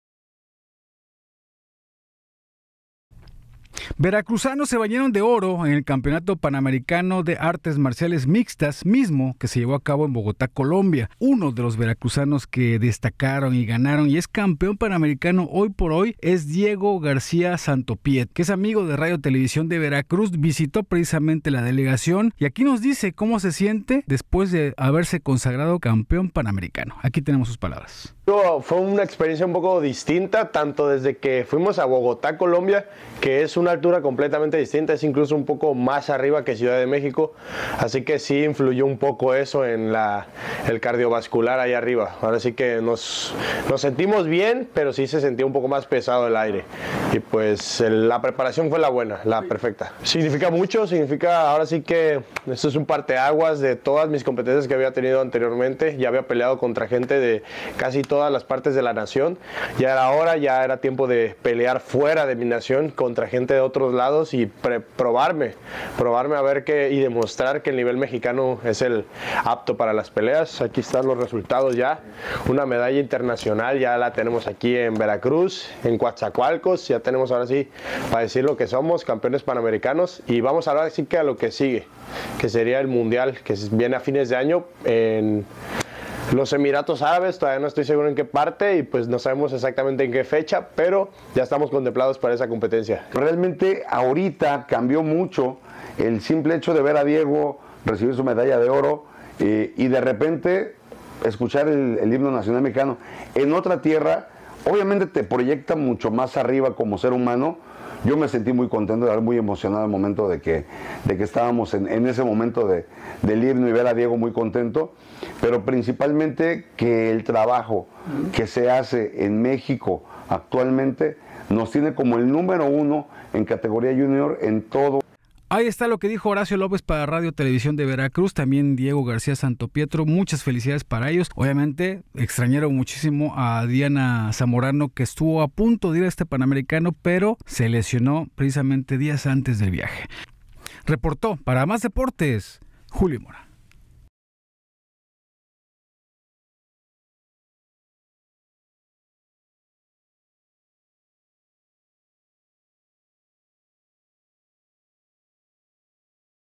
DIEGO SANTOPRIETO CAMPEON MMA
Diego García Santopietro es campeón Paramericano de Artes Marciales Mixtas en evento efectuado en Colombia.
Reportero: Julio Mora